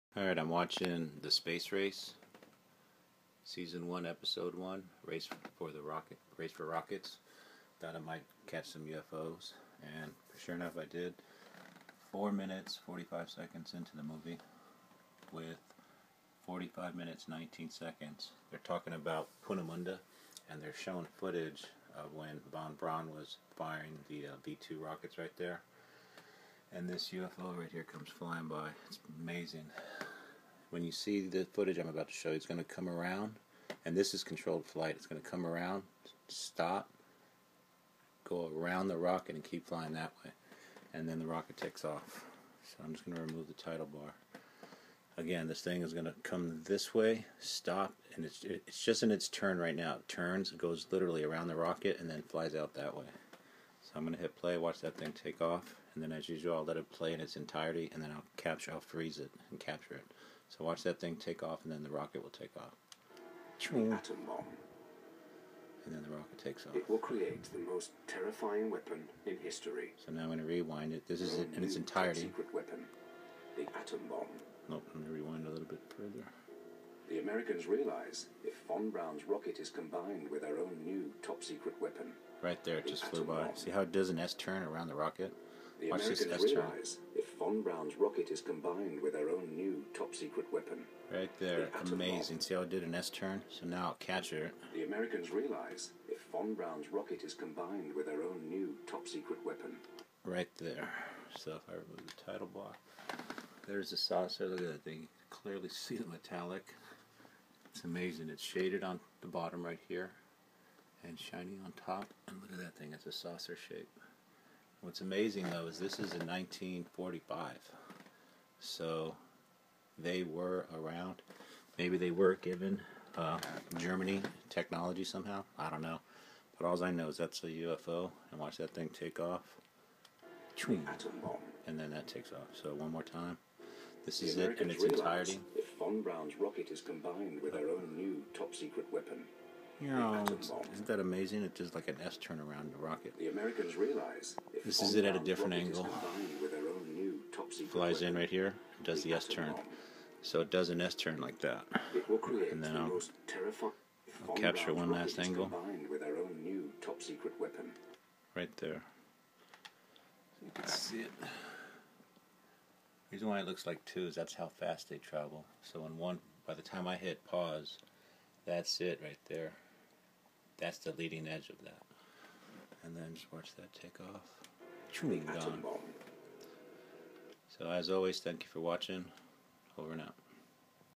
Exclusive! UFO Caught While V2 Rocket Launch Way Back In 1940's Some Of My Best Work
Usually I just let my videos speak for themselves because I narrate. This one is special because I was just watching TV and had nothing to do with ufo’s & saucers but every now and then they "masters of the universe " show themselves to me in one way or another in this case TV, why I don’t know! But I see them so I filmed it and put it here on YouTube for you to see what I am witnessing it is a gift from them this I know. In this video watch when the rocket launches from the left of the screen you will see the saucer fly into the screen the go around the rocket and they zip away. It’s amazing because this was filmed during Germany’s weapon testing program of the V-2 rocket at Peenemunde Army Research Center back in the 1940’s that is why this film is in black & white (very special to me). It taught me that these saucers or ufo’s same thing as far as I am concerned have been around forever.